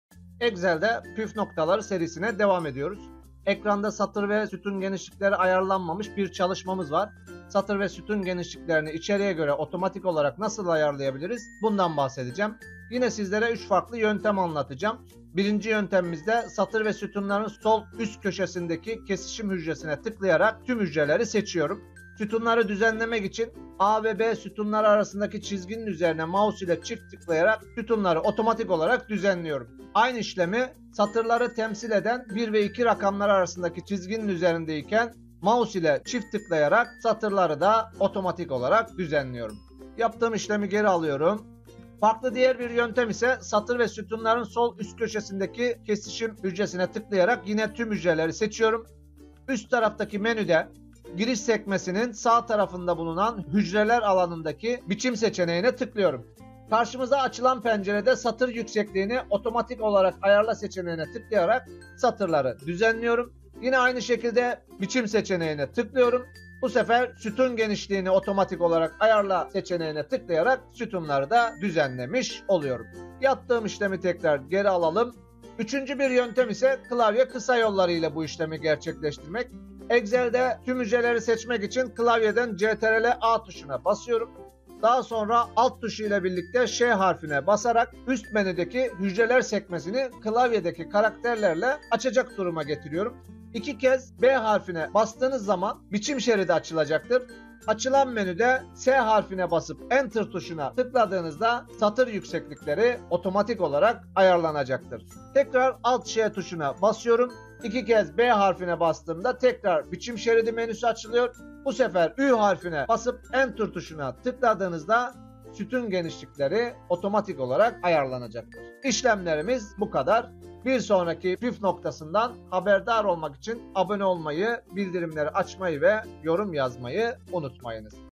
Excel Satır ve Sütun Genişliğini Ayarlamanın 3 Kolay Yolu! 📏 (Shorts)
Excel’de satır ve sütun genişliklerini ayarlamak artık çok daha kolay!
Bu kısa videoda sana, 3 farklı pratik yöntemle hücreleri nasıl daha düzenli ve okunaklı hale getireceğini gösteriyorum:
✔️ Otomatik boyutlandırma
✔️ Menülerden boyutlandırma
✔️ Klavye kısa yolları ile boyutlandırma

🧠 Profesyonel görünümlü tablolar için bu ipuçlarını kaçırma!